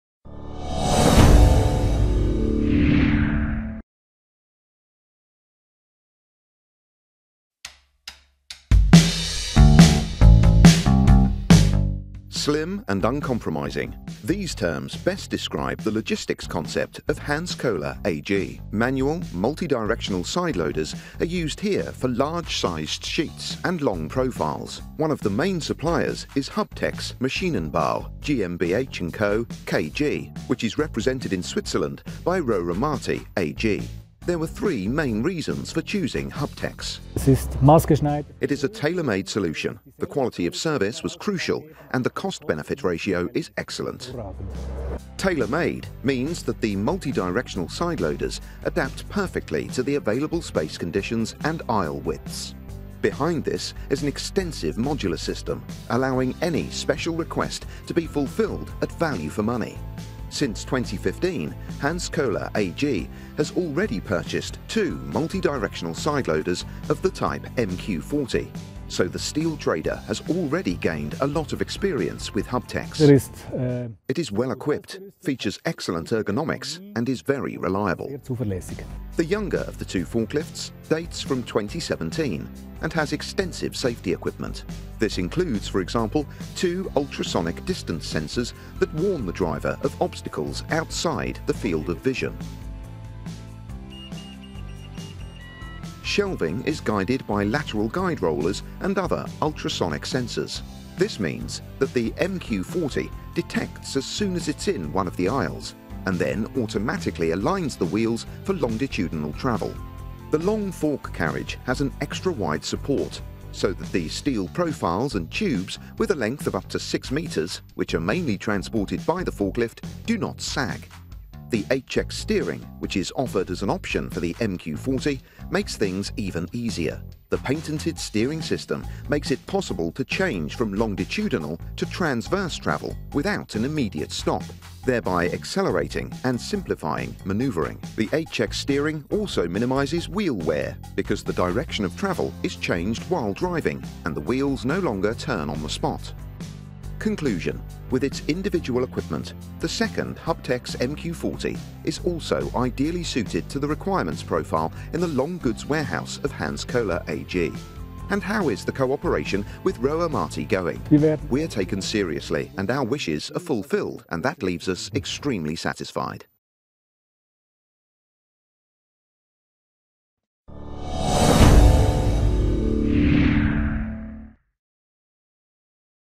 Logistics concept of Hans Kohler AG with HUBTEX multidirectional sideloaders
Since 2015, Hans Kohler AG has already purchased two multidirectional sideloaders of the type MQ40, so the steel trader has already gained a lot of experience with Hubtex.